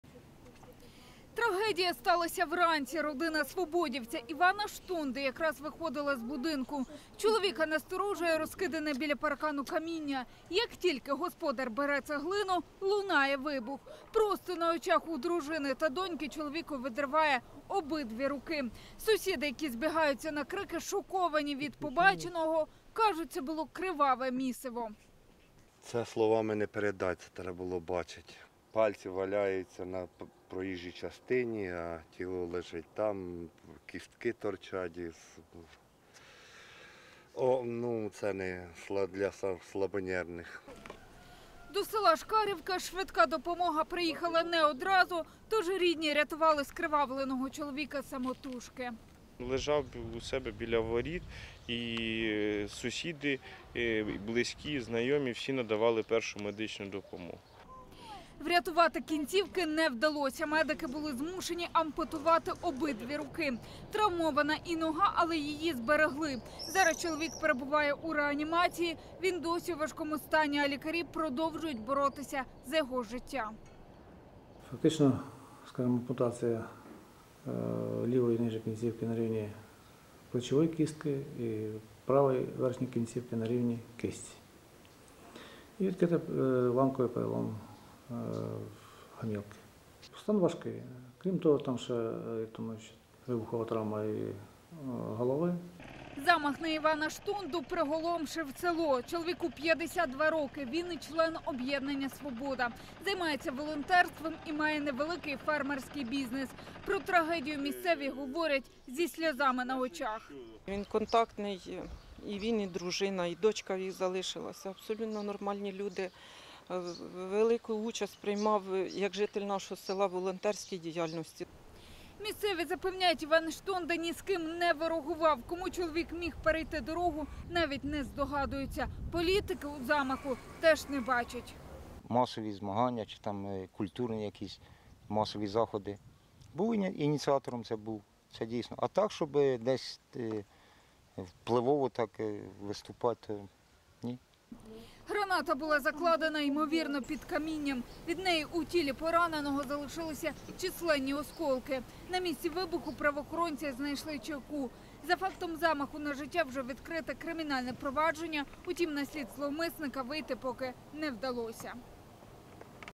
Свободівець залишився без обох рук через підкинуту гранату
Підступний замах! Свободівець Іван Штонда втратив обидві руки, отримав відкритий перелом лівої ноги, численні поранення і травми – унаслідок вибуху гранати, яку сховали у нього на подвір’ї. Медики борються за його життя і вже провели складну операцію.  З місця події повернулася Аліна Комарницька, вона знає подробиці.